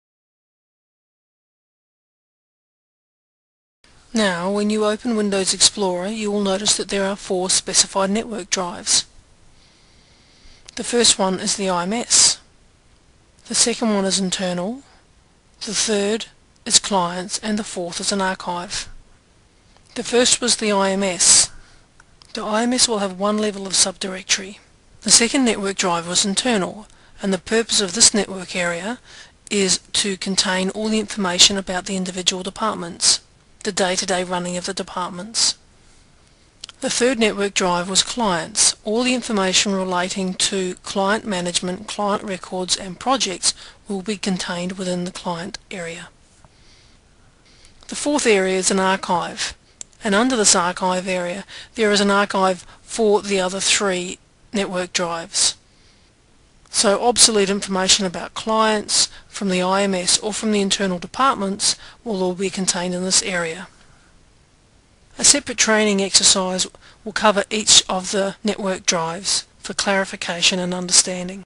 Training part 5 What is a data lode
There are four types of data or record created by any and every organization.  These need to form part of the Business Classification Scheme as part of their information governance.